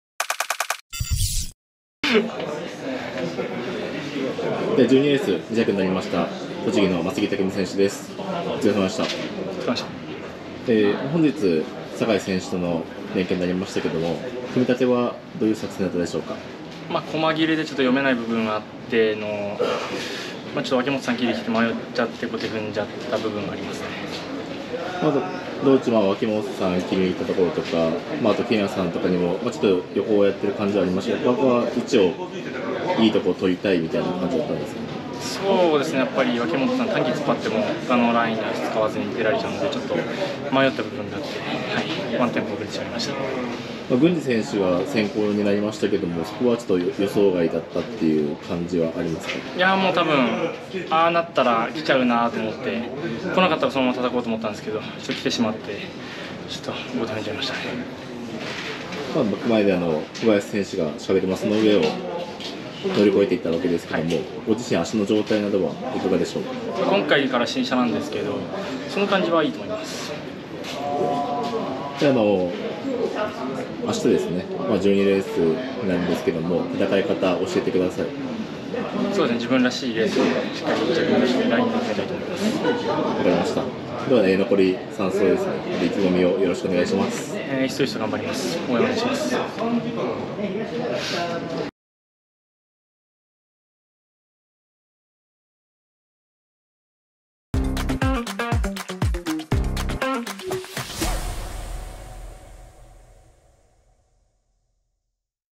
取手競輪水戸黄門賞GⅢ 眞杉匠(栃木・113期)初日S級特選 12R 2着
トリマクリでは取手競輪 水戸黄門賞GⅢのSPECIALCONTENTSを開設中！